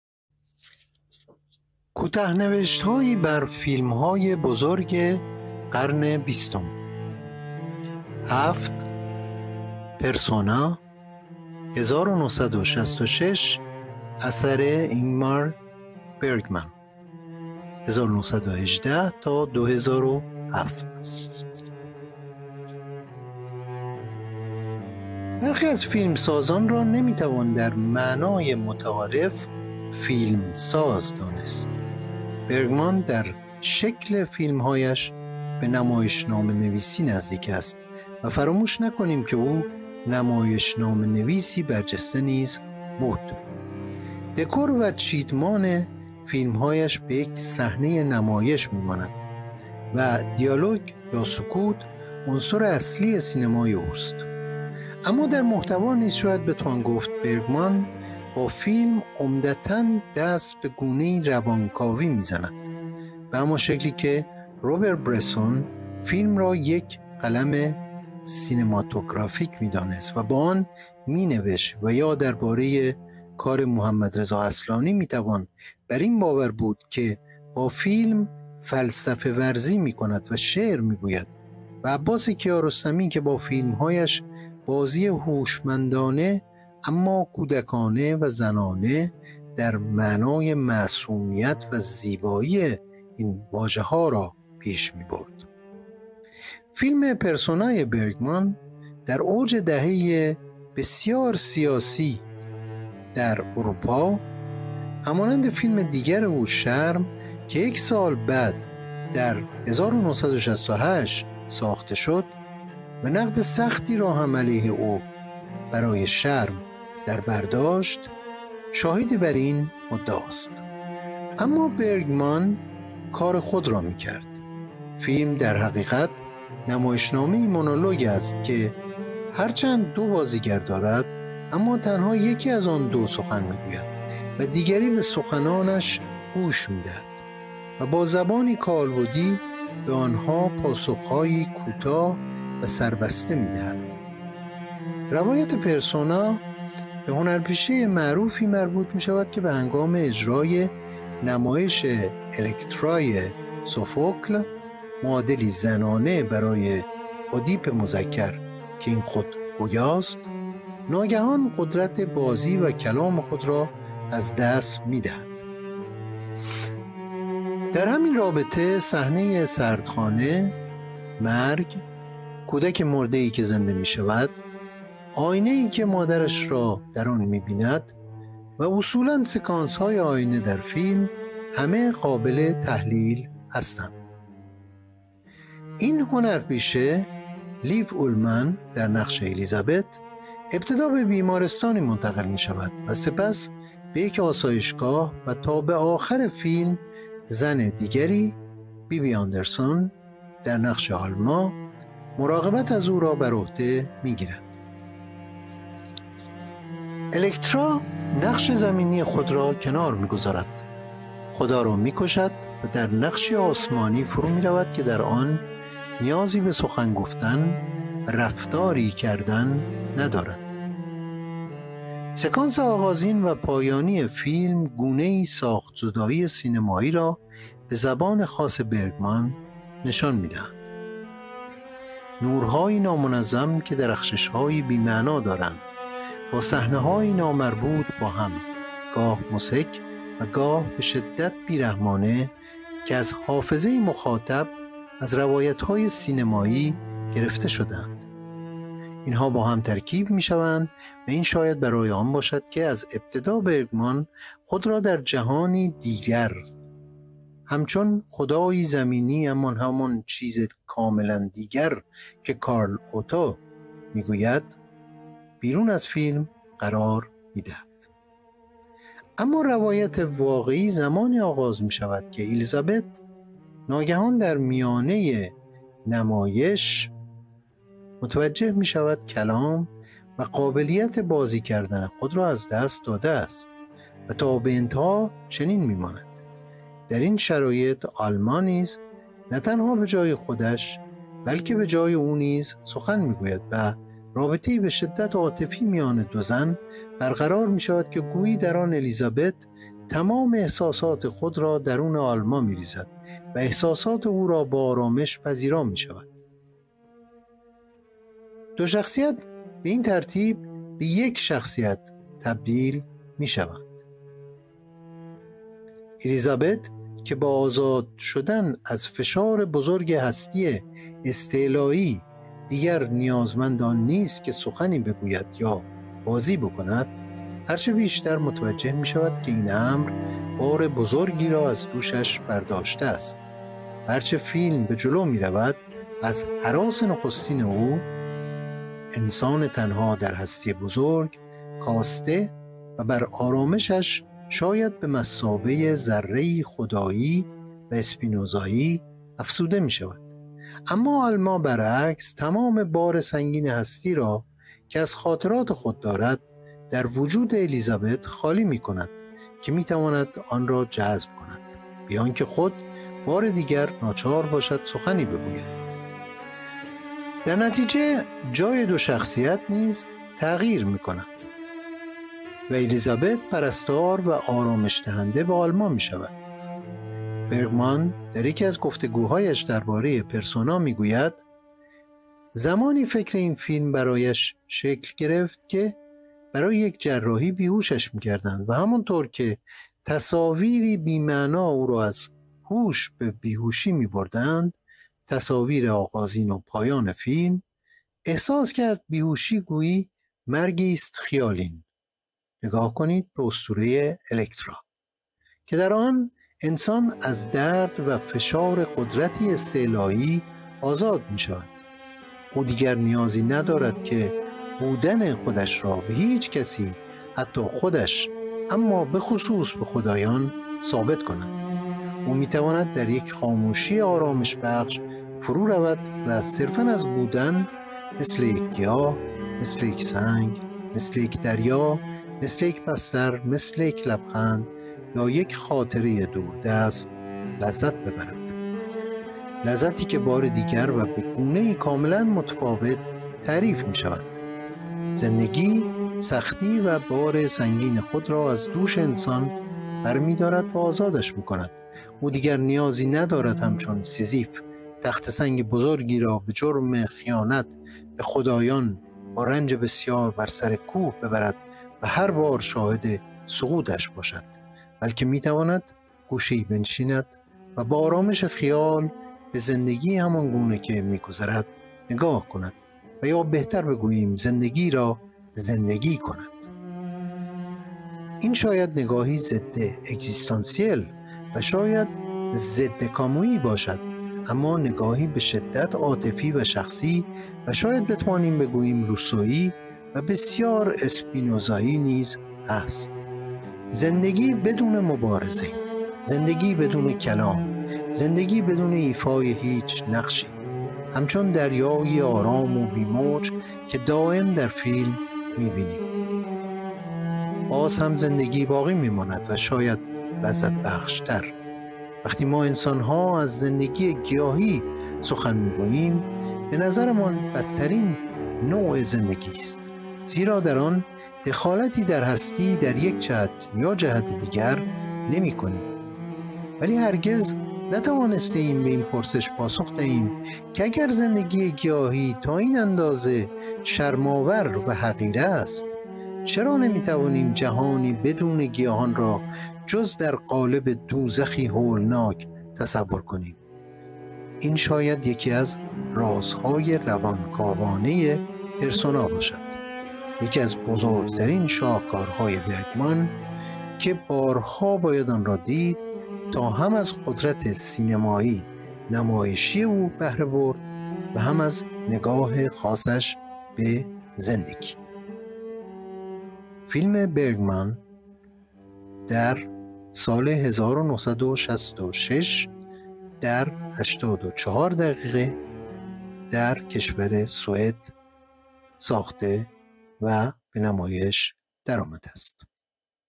کوته‌نوشت‌های سینما / هفت / پرسونا (1966) / N.Fakouhi/ Persona
کوته نوشت های سینما/ هفت/ پرسونا (۱۹۶۶)/ اینگمار برگمان (۱۹۱۸-۲۰۰۷)

برخی از فیلمسازان را نمی‌توان در معنای متعارف، «فیلم»‌ساز دانست. برگمان در «شکل» فیلم‌هایش به نمایشنامه‌نویسی نزدیک است و فراموش نکنیم که او نمایشنامه‌نویسی برجسته نیز بود. دکور و چیدمان فیلم‌هایش به یک صحنه نمایش می‌ماند و دیالوگ (یا سکوت) عنصر اصلی سینمای اوست. اما در محتوا نیز شاید بتوان گفت برگمان با فیلم، عمدتا دست به گونه‌ای روانکاوی می‌زند، به همان شکلی که روبر برسون، فیلم را یک قلم سینماتوگرافیک می‌دانست و با آن می‌نوشت و یا درباره کار محمدرضا‌ اصلانی می‌توان بر این باور بود که با فیلم، فلسفه‌ورزی می‌کند و شعر می‌گوید و عباس کیارستمی که با فیلم‌هایش بازی هوشمندانه اما کودکانه‌ و زنانه (در معنای معصومیت و زیبایی این واژه) پیش می‌برد. فیلم پرسونای برگمان در اوج دهه‌ای بسیار سیاسی در اروپا، همانند فیلم دیگر او «شرم» که یک سال بعد، در ۱۹۶۸ ساخته شد و نقد سختی را هم علیه او (برای «شرم») در بر داشت، شاهدی بر این مدعاست. اما برگمان کار خود را می‌کرد. فیلم در حقیقت، نمایشنامه‌ای مونولوگ است که هر‌چند دو بازیگر دارد، اما تنها یکی از آن دو سخن می‌گوید و دیگری به سخنانش گوش می‌دهد و با زبانی کالبدی به آن‌ها پاسخی کوتاه و سربسته می‌دهد. روایت پرسونا، به هنرپیشه معروفی مربوط می‌شود که به هنگام اجرای نمایش «الکترا»ی سوفوکل (معادلی زنانه برای «اودیپ» مذکر، که این خود گویاست) ناگهان قدرت بازی و کلام خود را از دست می‌دهد.در همین رابطه  صحنه سردخانه، مرگ، کودک مرده که زنده می‌شود، آینه‌ای که مادرش را می بیند و اصولا سکانس‌های آینه در فیلم همه قابل تحلیل هستند.  این هنرپیشه (لیو اولمن در نقش الیزابت) ابتدا به بیمارستانی منتقل می‌شود و سپس به یک آسایشگاه و تا به آخر فیلم، زن دیگری (بی‌بی آندرسون در نقش آلما) مراقبت از او را برعهده می‌گیرد. الکترا نقش زمینی خود را کنار می گذارد ، خدا را می کشد و در نقشی آسمانی فرو می‌رود که در آن نیازی به سخن گفتن و رفتاری کردن ندارد. سکانس آغازین و پایانی فیلم گونه‌ای «ساخت‌زدایی» سینمایی را به زبان خاص برگمان نشان می‌دهند: نورهایی نامنظم که درخشش‌هایی بی‌معنا دارند با صحنه‌هایی  نا مربوط با هم، گاه مضحک و گاه به شدت بی‌رحمانه که از حافظه مخاطب از روایت‌های سینمایی گرفته شده‌اند، ترکیب می‌شوند و این شاید برای آن باشد که از ابتدا برگمان خود را در جهانی «دیگر» (خدایی زمینی اما همان «چیز کاملا دیگر» کارل اوتو) بیرون از فیلم قرار دهد. اما روایت واقعی زمانی آغاز می‌شود که الیزابت ناگهان در میانه نمایش متوجه می‌شود «کلام» و قابلیت «بازی کردن» خود را از دست داده است و تا به انتها چنین می‌ماند. در این شرایط، آلما، نه تنها به جای خودش بلکه به جای او نیز سخن می‌گوید و رابطه‌ای به شدت عاطفی میان دو زن برقرار می‌شود که گویی در آن الیزابت، تمام احساسات خود را درون آلما می‌ریزد و احساسات او را با آرامش پذیرا می‌شود. دو شخصیت به این ترتیب به یک شخصیت تبدیل می‌شوند. الیزابت که با آزاد شدن از فشار بزرگ هستی استعلایی، دیگر نیازمند آن نیست که سخنی بگوید یا «بازی» بکند، هرچه بیشتر متوجه می‌شود که این امر، بار بزرگی را از دوشش برداشته است و هر‌چه فیلم به جلومی رود، از هراس نخستین او (انسان تنها در هستی بزرگ) کاسته و بر آرامشش شاید به مثابه «ذره خدایی» اسپینوزایی افزوده می‌شود. اما آلما برعکس، تمام بار سنگین هستی را که از خاطرات خود دارد، در وجود الیزابت خالی می‌کند که می‌تواند آن را جذب کند، بی‌آنکه خود بار دیگر ناچار باشد سخنی بگوید. در نتیجه، جای دو شخصیت نیز تغییر می‌کند و الیزابت پرستار و آرامش‌دهنده به آلما می‌شود. برگمان در یکی از گفتگوهایش درباره پرسونا می‌گوید: زمانی فکر این فیلم برایش شکل گرفت که برای یک جراحی بیهوشش می‌کردند و همان‌طور که تصاویری بی‌معنا او را از هوش به بیهوشی می‌بردند (تصاویر آغازین و پایان فیلم) احساس کرد بیهوشی گویی مرگی است خیالین (نگاه کنید به اسطوره الکترا) که در آن، انسان از درد و فشار قدرتی استعلایی آزاد می‌شود: او دیگر نیازی ندارد که «بودن» خودش را به هیچ کسی، حتی خودش، اما به خصوص به  خدایان، ثابت کند؛ او می‌تواند در یک خاموشی ِ آرامش‌بخش فرو رود و صرفا از «بودن» مثل یک گیاه، مثل یک سنگ، مثل یک دریا، مثل یک بستر، مثل یک لبخند، یا یک خاطره دوردست، لذت ببرد.
لذتی که بار دیگر و به گونه‌ای کاملا متفاوت تعریف می‌شود: زندگی، سختی و بار سنگین خود را از دوش‌ انسان بر می‌دارد و آزادش می‌کند او دیگر نیازی ندارد همچون سیزیف تخته سنگ بزرگی را به جرم خیانت به خدایان  با رنج بسیار بر سرکوه ببرد و هر بار شاهد سقوطش باشد، بلکه می‌تواند گوشه‌ای بنشیند،  و با آرامش خیال به زندگی همان‌گونه که می‌گذرد نگاه کند و یا بهتر بگوییم زندگی را زندگی کند: این شاید نگاهی ضد اگزیستانسیل و شاید ضد کامویی باشد اما نگاهی به شدت عاطفی و شخصی و شاید بتوانیم بگوییم روسویی و بسیار اسپینوزایی نیز هست. زندگی بدون مبارزه، زندگی بدون کلام، زندگی بدون ایفای هیچ نقشی، همچون دریایی آرام و  بی‌موج که دائم در فیلم می‌بینیم، بازهم زندگی باقی می‌ماند و شاید لذت بخش‌تر. وقتی ما انسان‌ها از «زندگی گیاهی» سخن می‌گوییم، به نظرمان بدترین زندگی است زیرا در آن دخالتی در هستی  در یک جهت یا جهت دیگر نمی‌کنیم، ولی هرگز نتوانسته‌ای به این پرسش پاسخ دهیم که اگر «زندگی گیاهی» تا این اندازه  شرم‌آور و حقیر است، چرا نمی توانیم جهان بدون گیاهان را جز در قالب دوزخی هولناک تصور کنیم؟  این شاید یکی از راز‌های روانکاوانه پرسونا باشد: یکی از بزرگترین شاهکارهای برگمان که بارها باید آن را دید تا هم از قدرت سینمایی/ نمایش او بهر برد و هم از نگاه خاصش به زندگی.